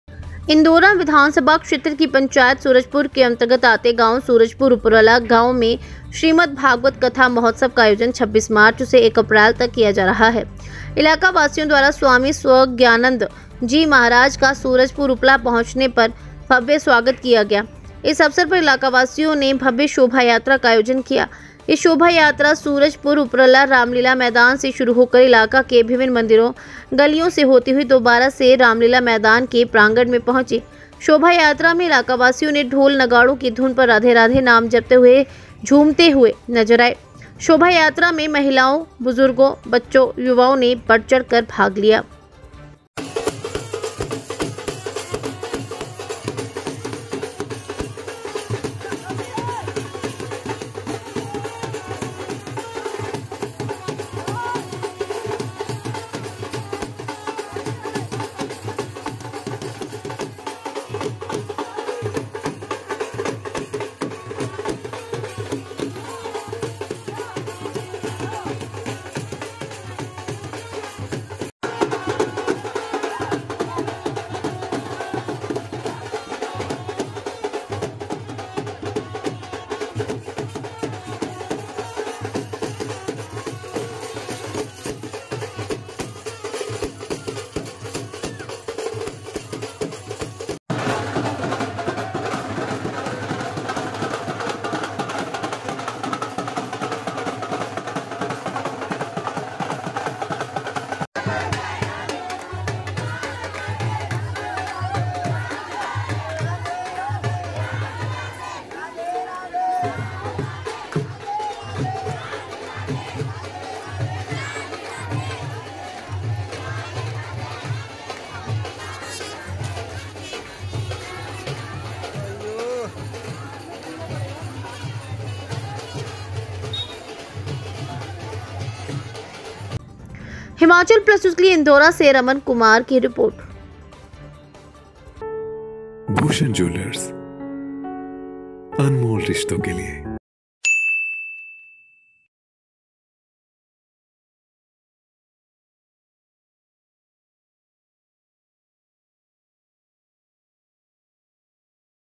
Himachal News | Surajpur में मनाया जा रहा श्रीमद्भागवत कथा महोत्सव
Indora: Shrimad Bhagwat Katha Mahotsav being celebrated in Surajpur

Keep Sharing

Keep Supporting